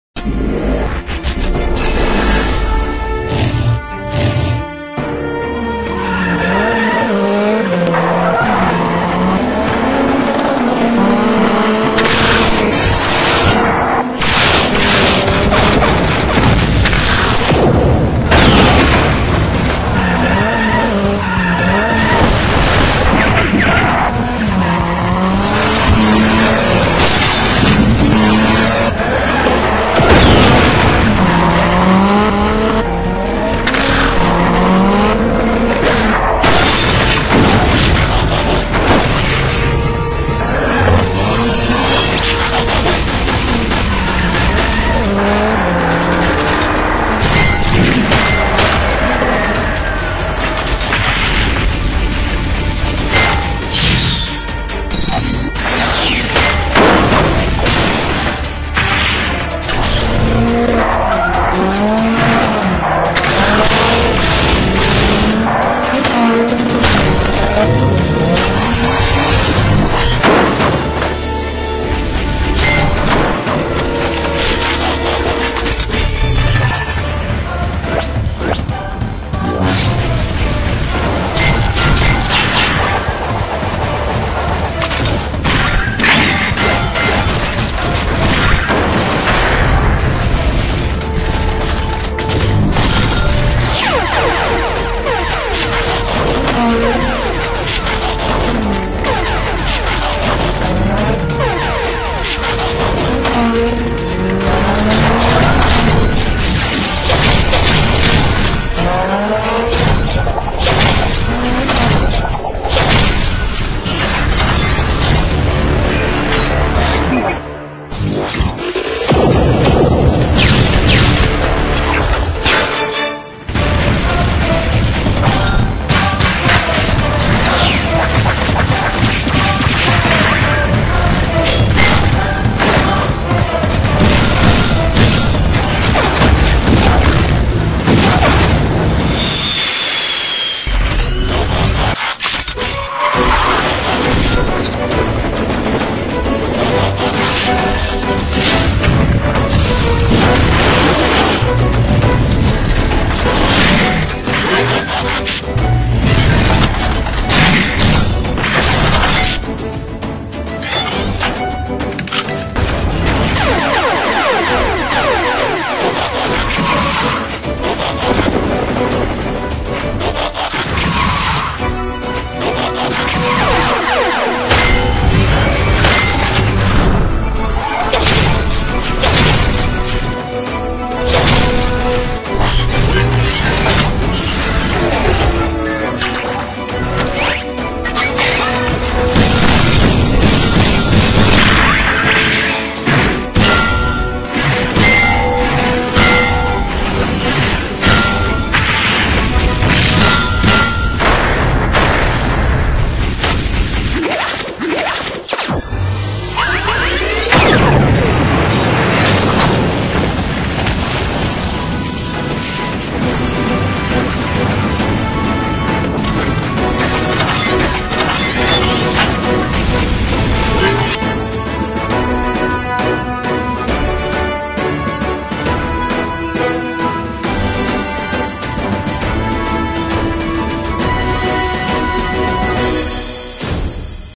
Transformers Stop motion - Prime VS Menasor 柯博文VS飛天虎
Optimus Prime in Action again,  Prime will face a new big enemy this time, video took around 20 days of production time and 3000 pictures.